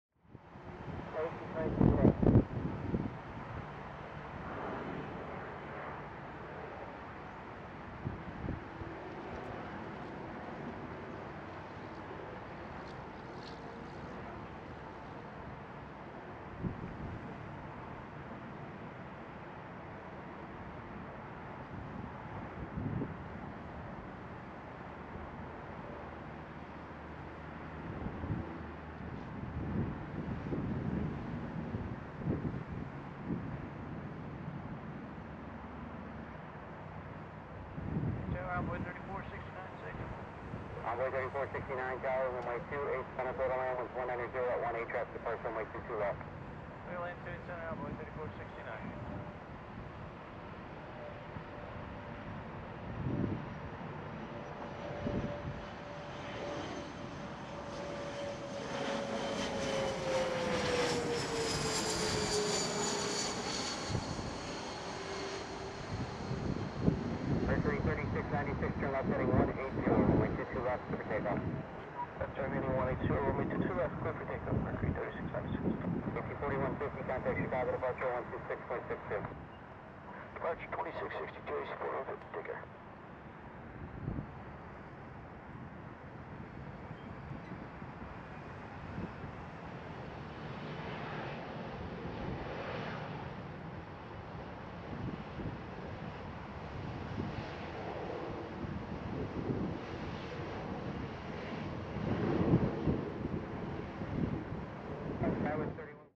Dangerous Crosswind Landing CRJ - Nose Wheel First - American Eagle - Chicago O'Hare Plane Spotting
Amazing footage of this Dangerous Crosswind Landing of this American Eagle CRJ. The nose wheel touched first before the main gears. Chicago O'Hare Plane Spotting during afternoon hours.